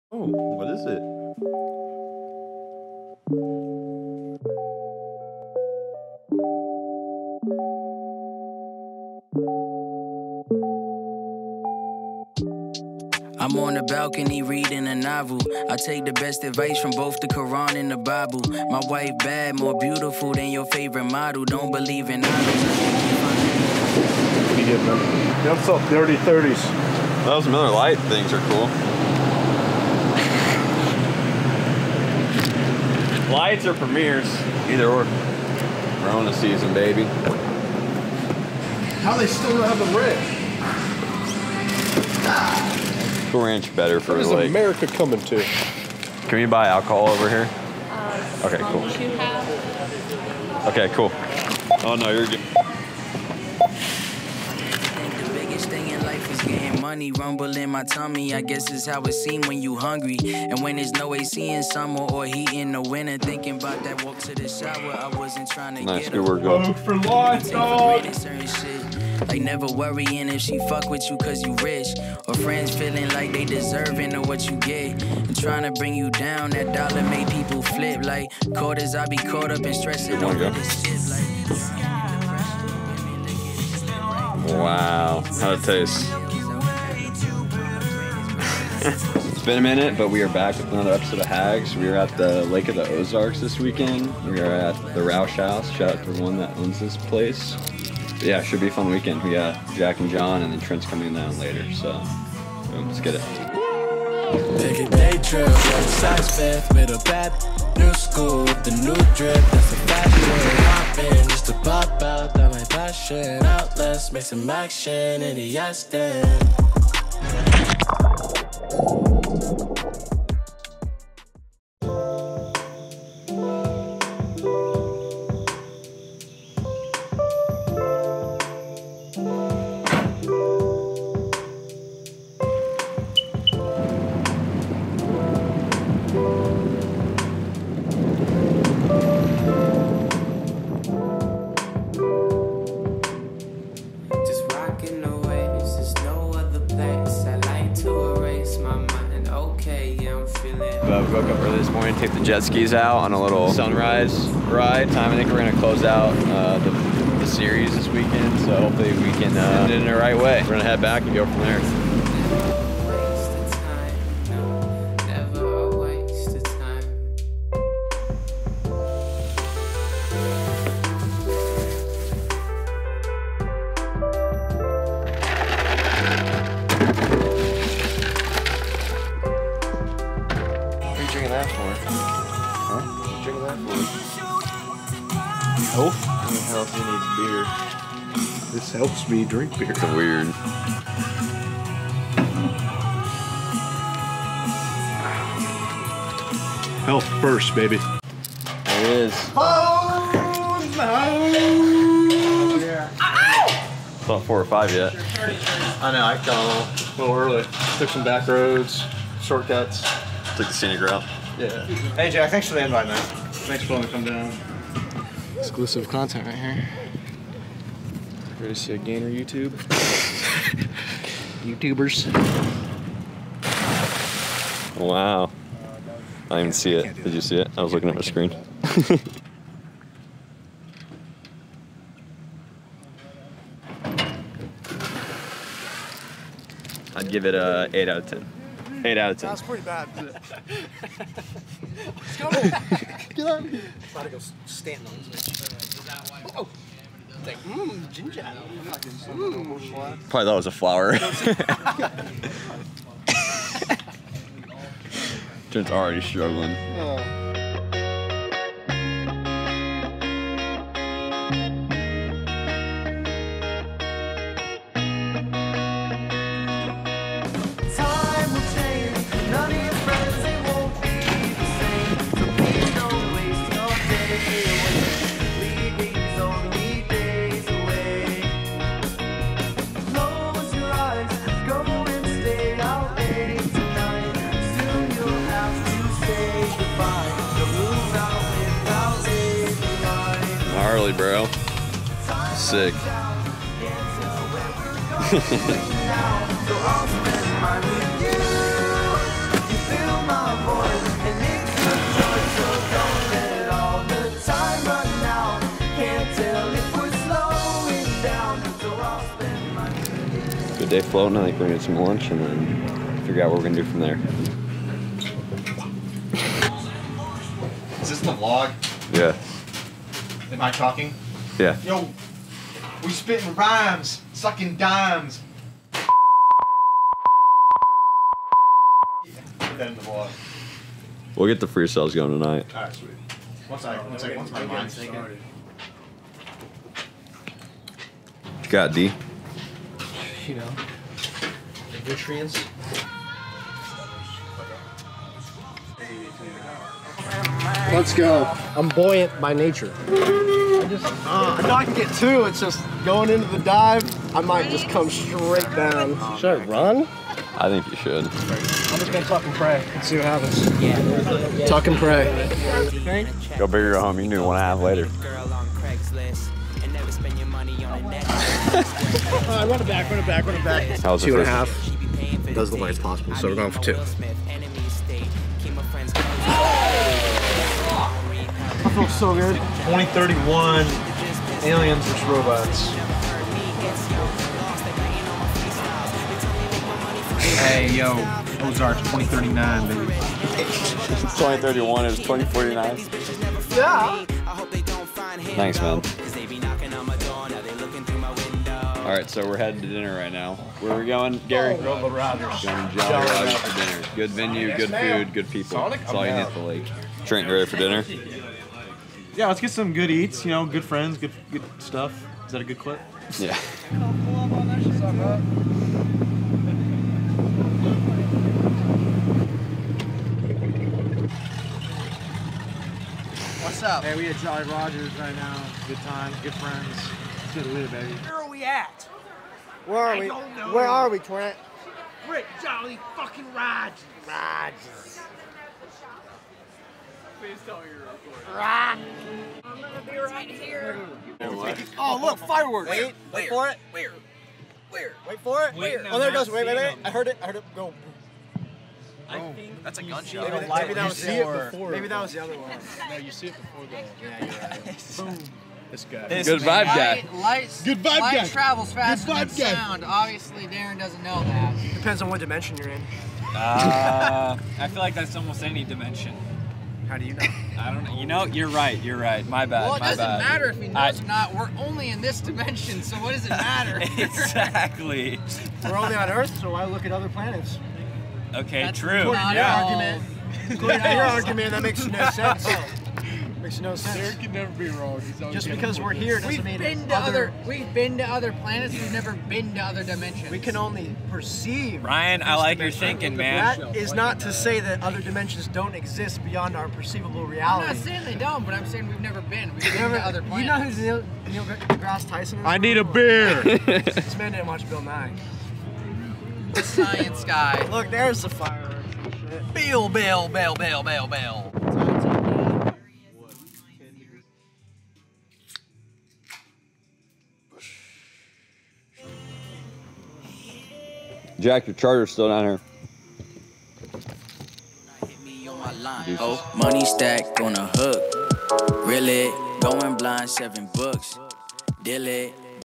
Sunrise Jet Ski Ride
No better way to end the summer series than a weekend at the lake. Thanks to everyone who has been following along with H.A.G.S. 🌊

Shoutout Tolu for the Intro music. The kid is crazy talented so give him a listen